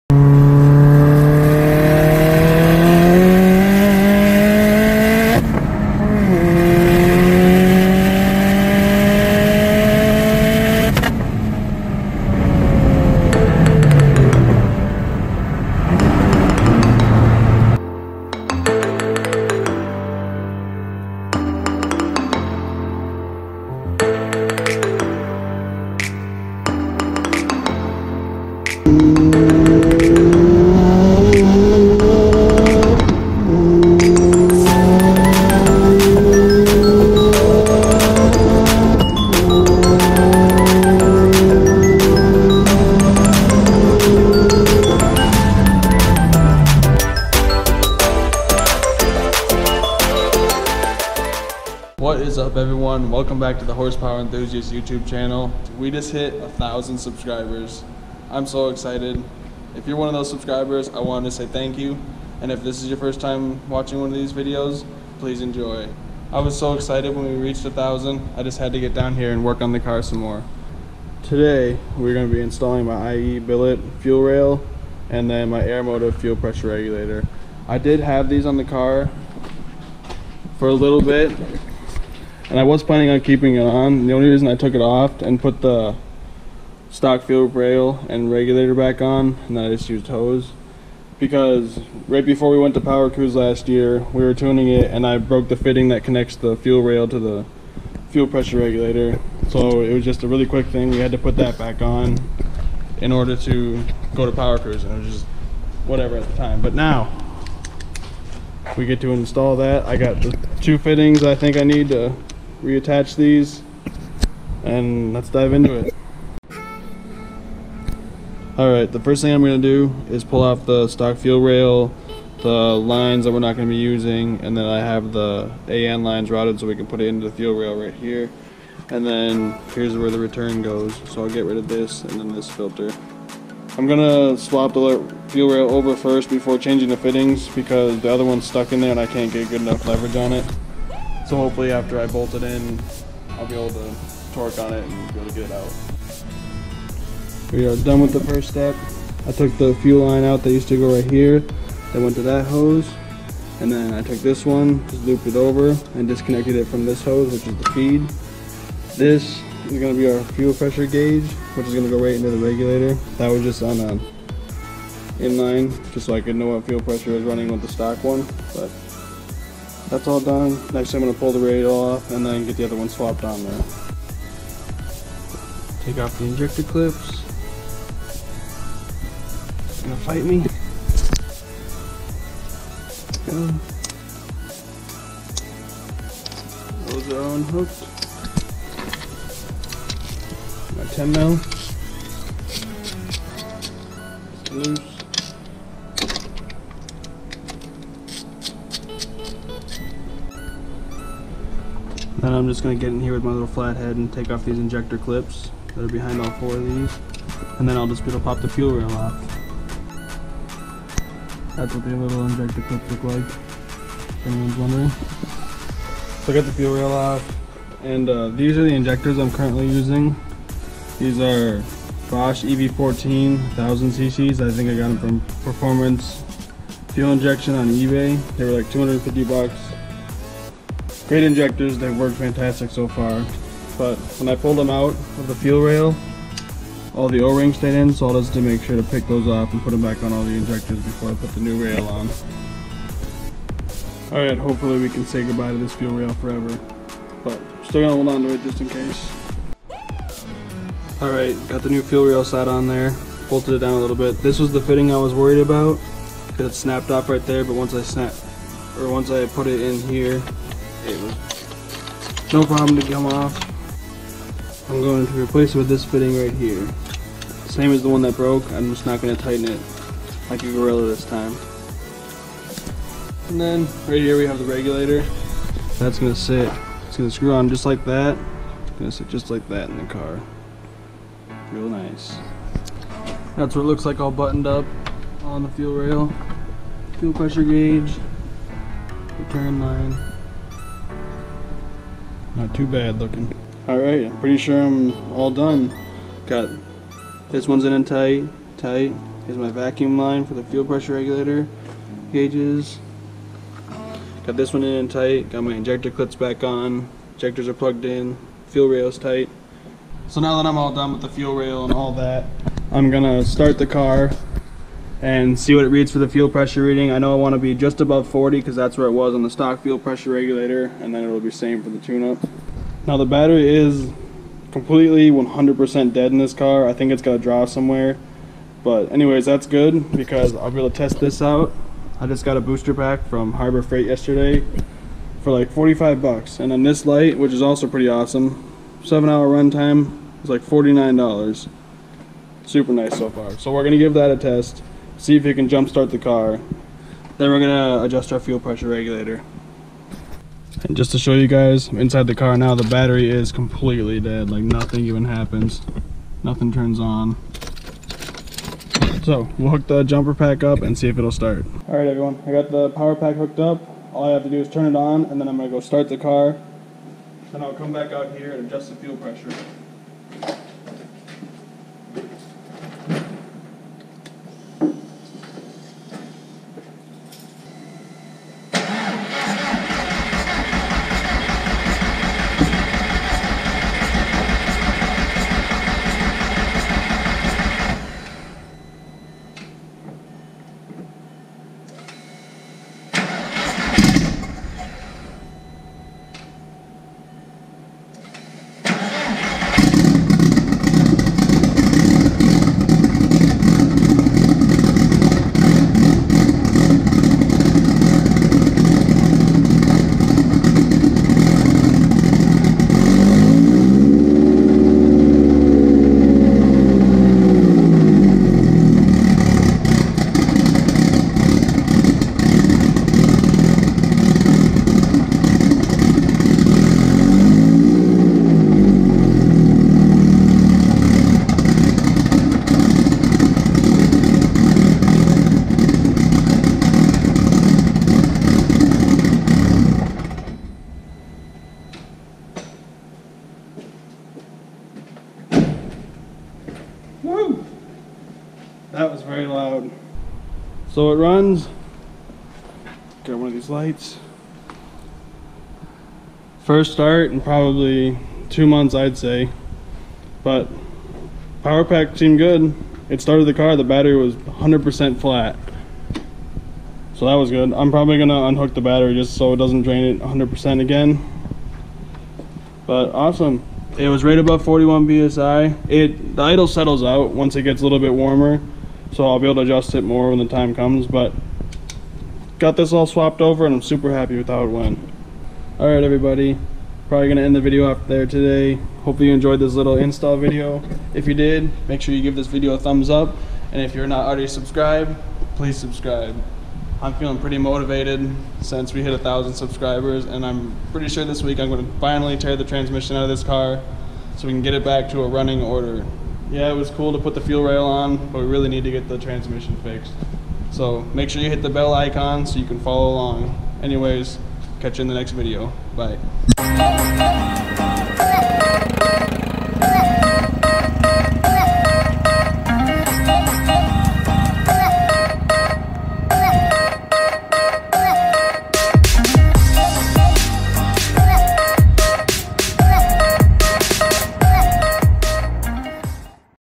500hp Jetta IE billet fuel rail install!
Just a little install video of my billet IE fuel rail and aeromotive fuel pressure regulator!

haha but seriously for some parts